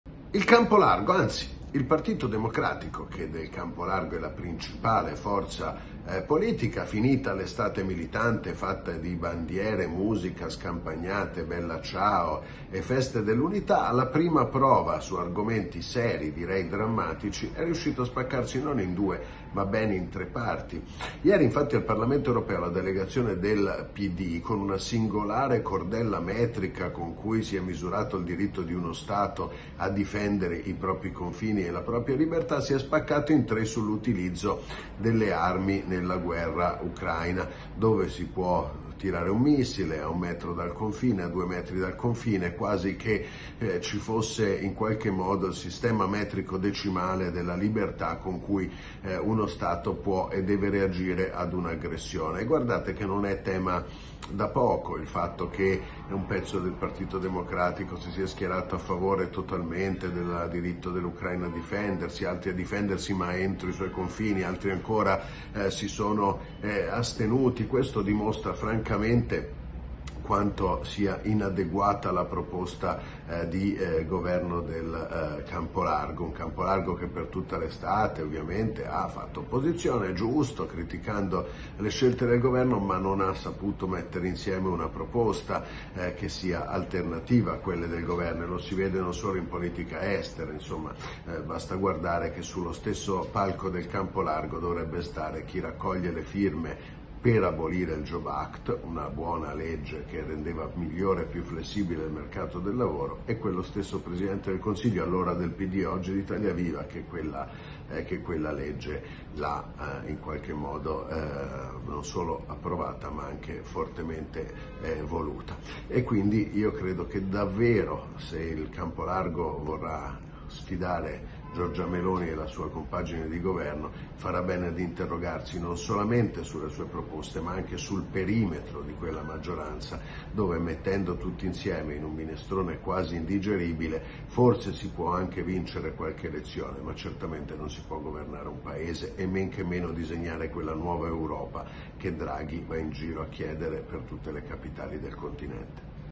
Ucraina, sinistra spaccata: alla prima prova si sfalda il campo largo
Dopo un'estate di militanza, il campo largo si è già sfaldato. Il Partito democratico, che ne è la forza principale, si è spaccato in tre parti alla prima prova, quella del voto del Parlamento europeo sulla rimozione del veto all'Ucraina per l'utilizzo delle armi occidentali contro obiettivi in territorio russo